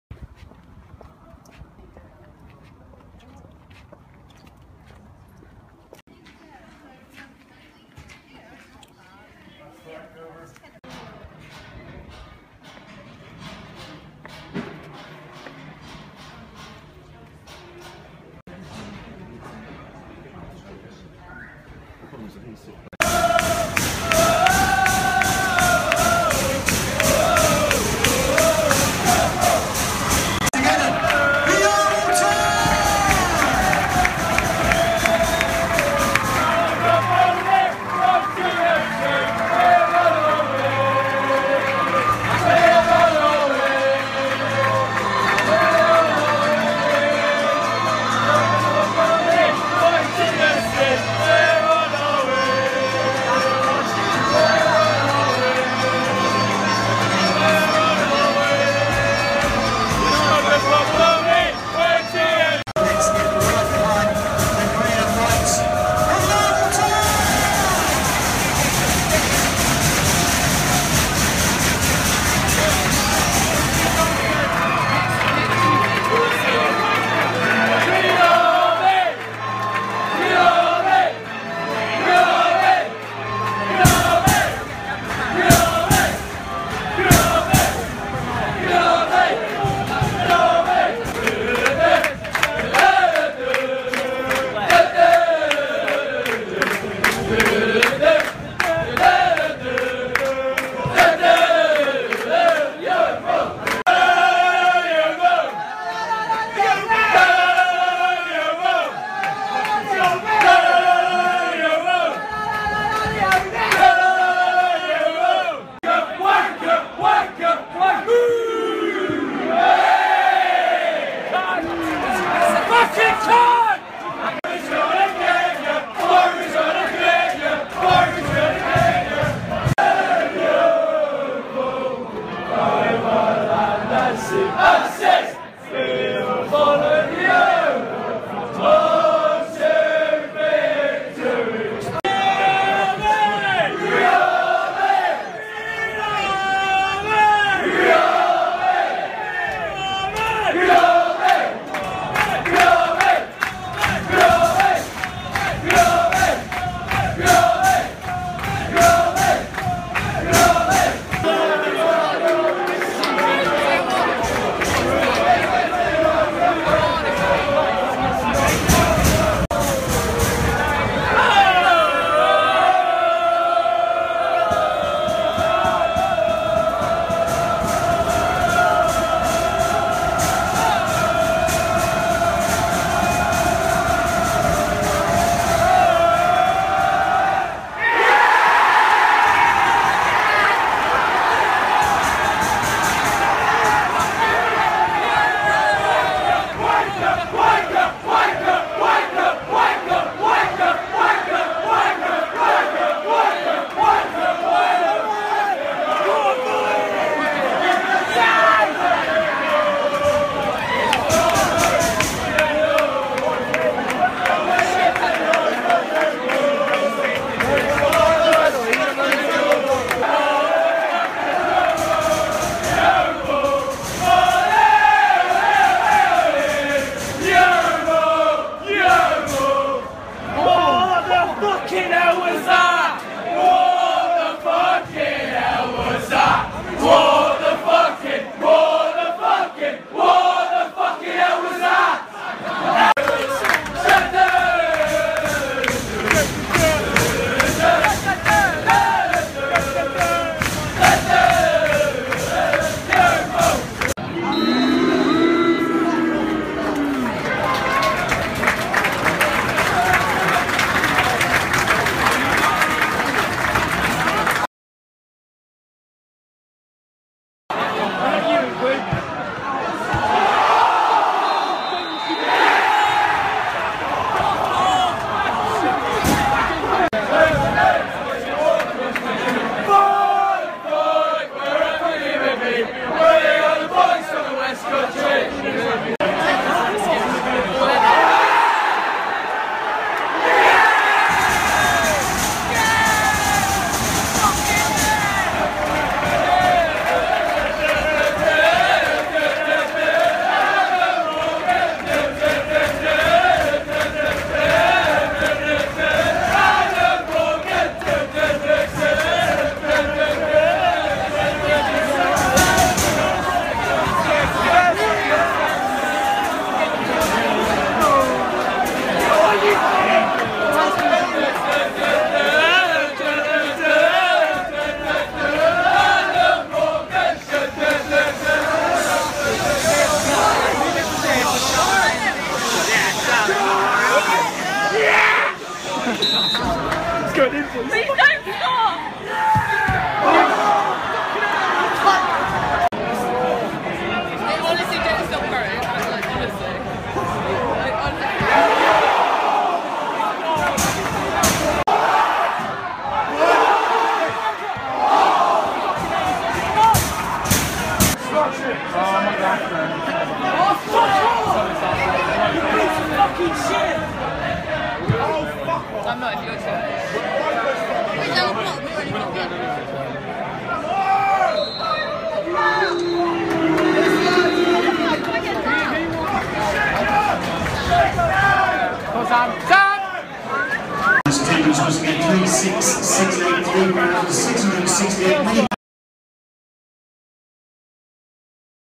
YTFC fans vs Walsall
Yeovil 0-1 Walsall

A poor Town display sees us drop further away from survival. The winning goal came from ex glover Jordi Hiwula